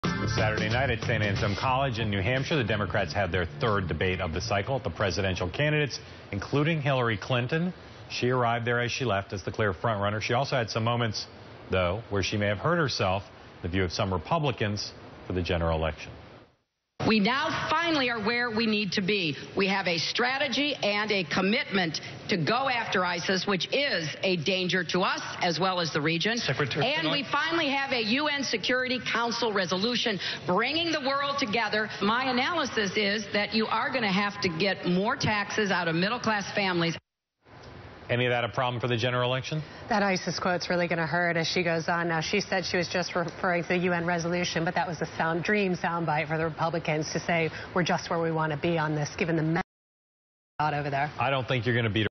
Bloomberg’s Megan Murphy On Clinton's ISIS Debate Moment: “Really Going To Hurt”
Bloomberg’s Megan Murphy On Clinton's ISIS Debate Moment: “Really Going To Hurt” (December 21, 2015)